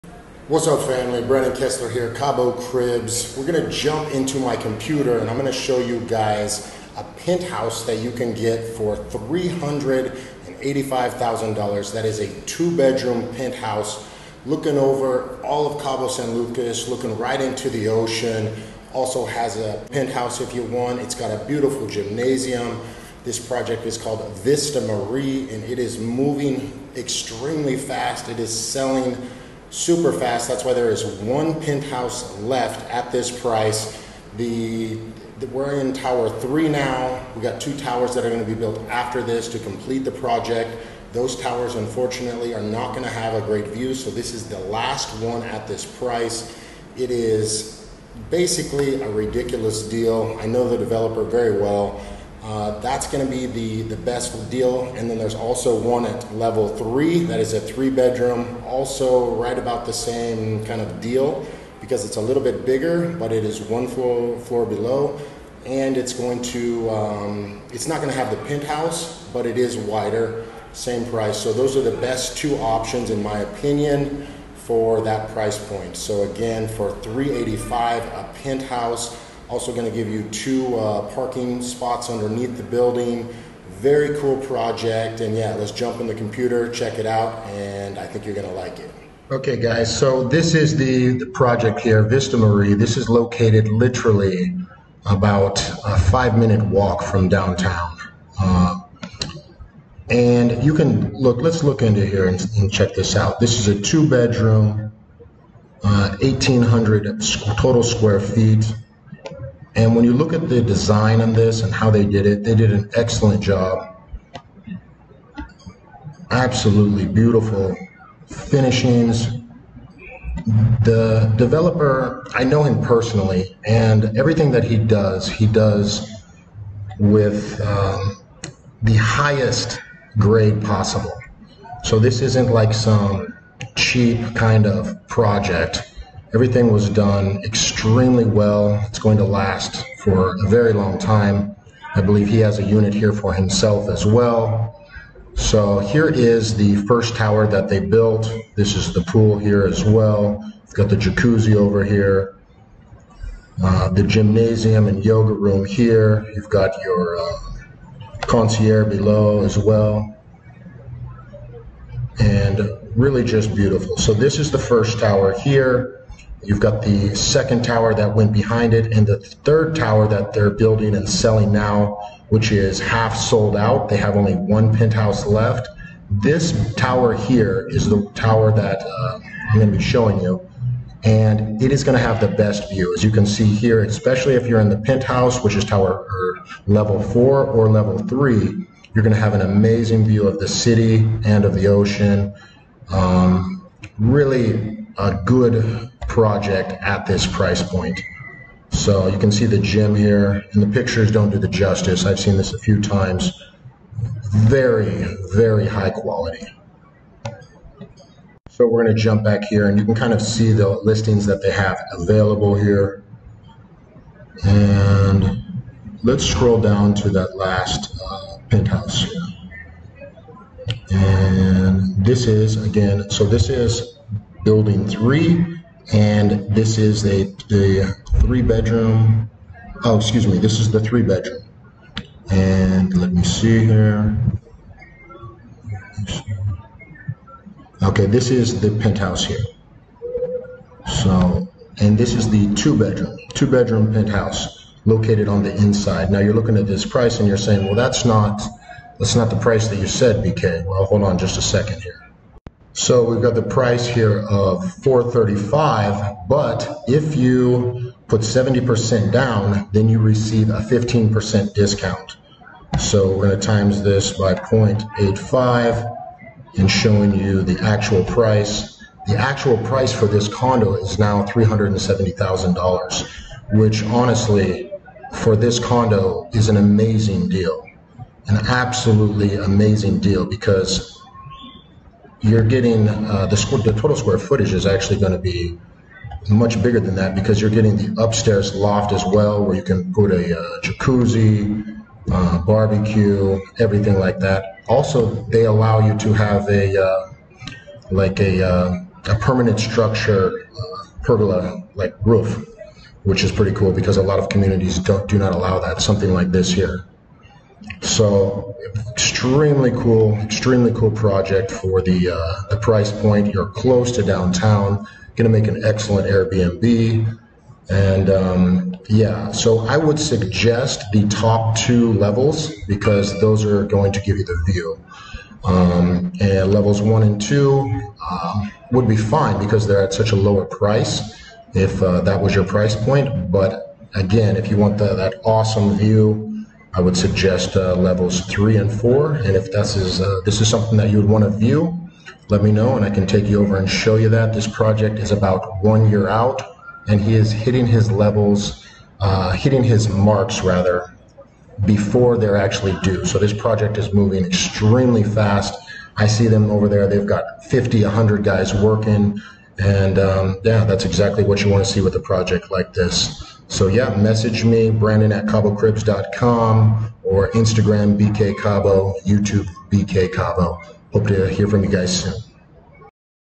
Cabo Penthouse | Vista Mare $369,000 USD
Vista Mare Cabo Condos starting just over $200,000 in Downtown Cabo San Lucas. This is an amazing deal for and investment in Cabo or for an AirBnB in Cabo!